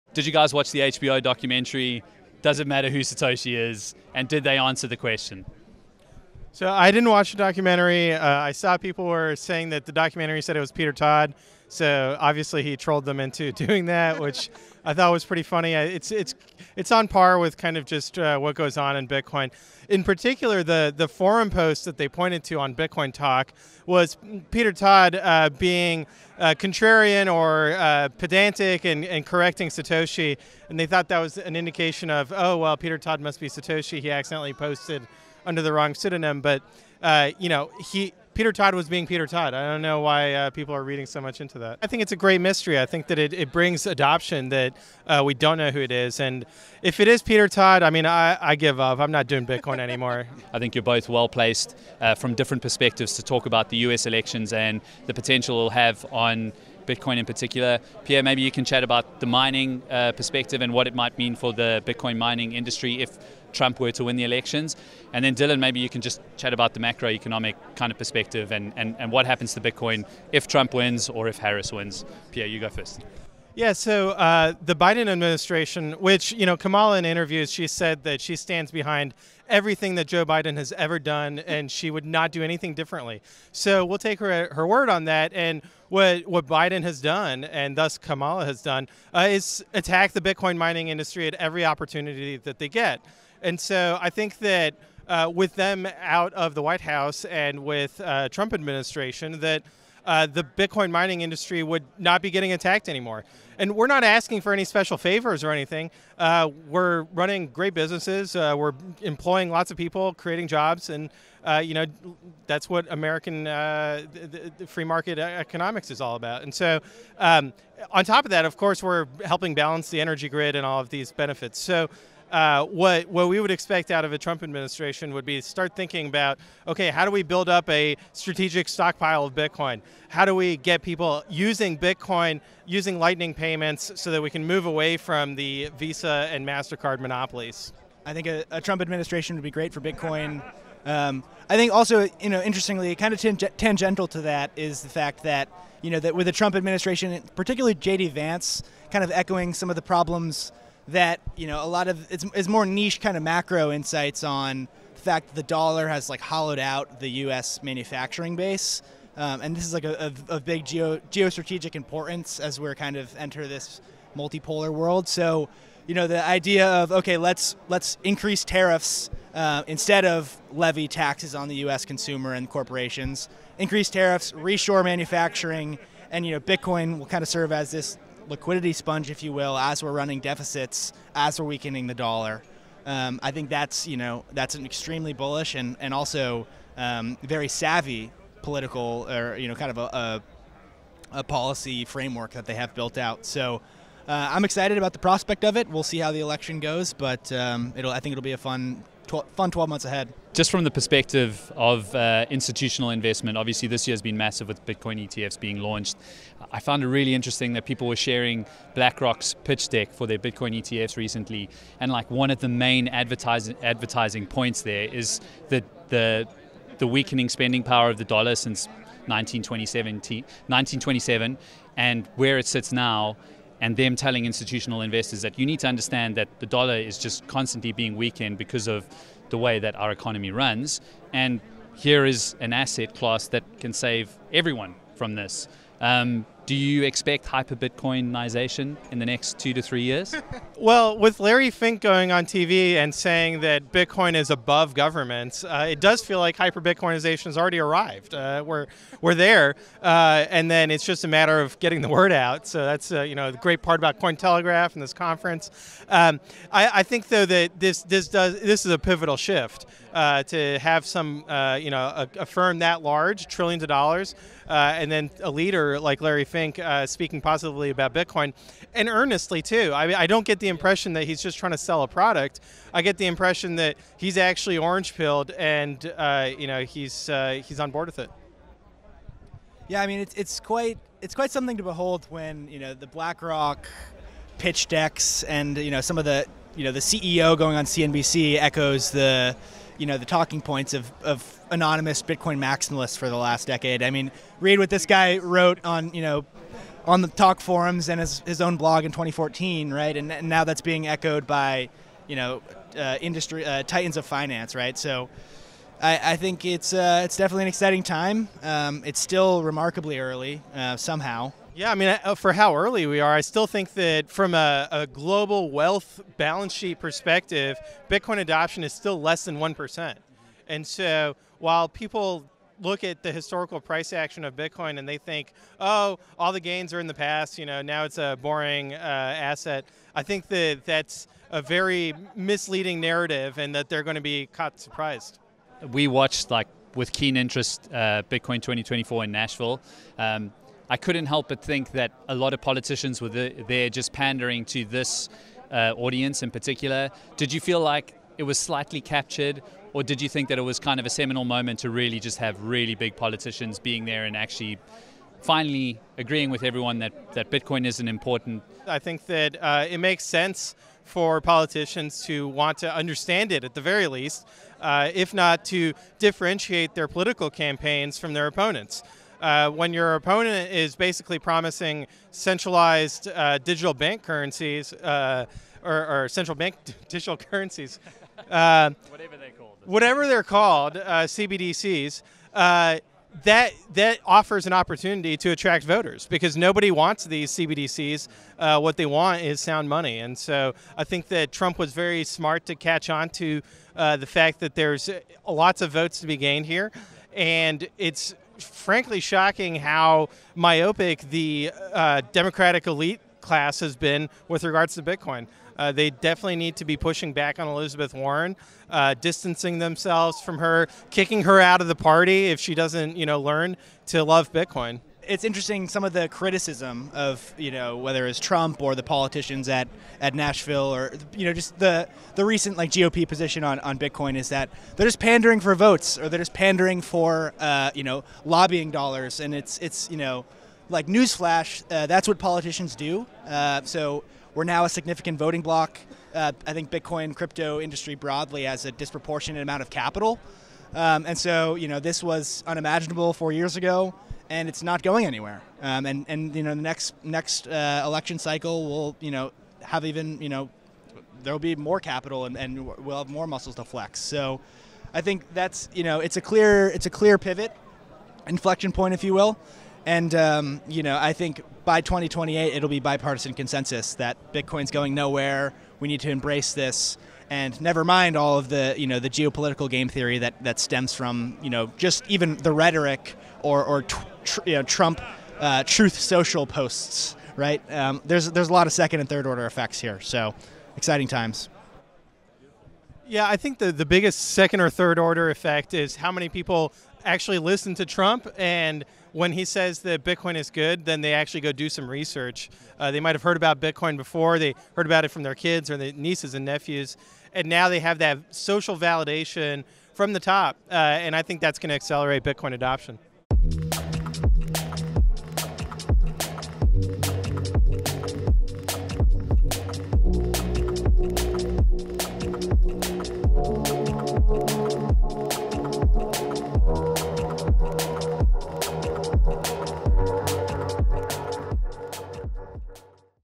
Who is better for the crypto industry - Trump or Kamala?
While we were at Bitcoin Amsterdam, we caught up with Pierre Rochard, VP of research at Riot Platforms and Dylan LeClair, institutional lead at Bitcoin Magazine, to ask them about their thoughts on the recently released HBO documentary in which they named Satoshi Nakamoto. We also ask them about the upcoming US elections, who they think will be better for the crypto industry, Trump or Kamala, is hyperbitcoinization already here and if politicians actually agree that Bitcoin is important.